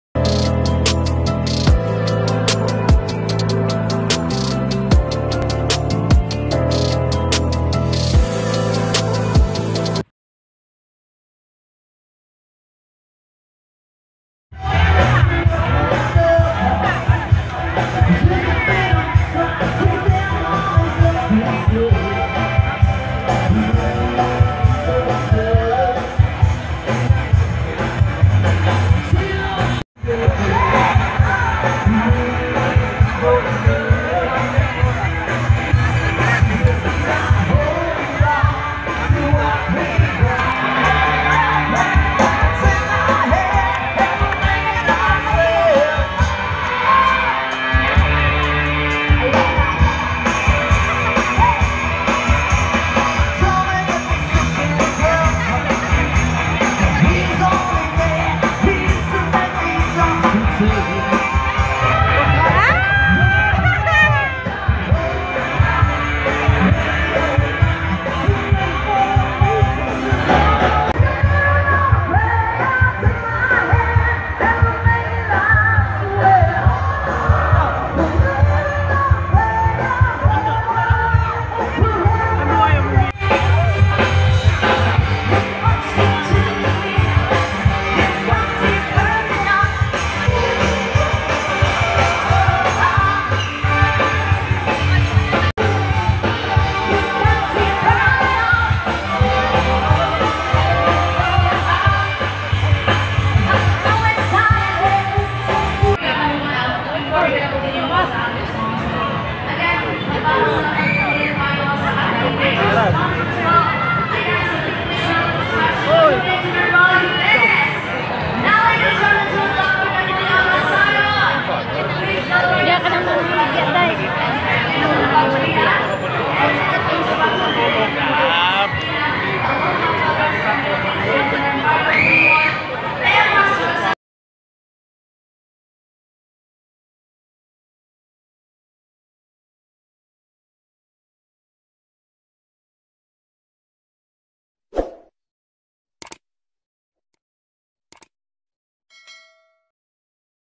Friday Chill Night II Live Band II Capilay Spring Park
Hey Guys..it's finally friday,,here's a short video..sharing to you on what our friday night looks like during weekend...just this month of August,,in preparation of the upcoming town fiesta.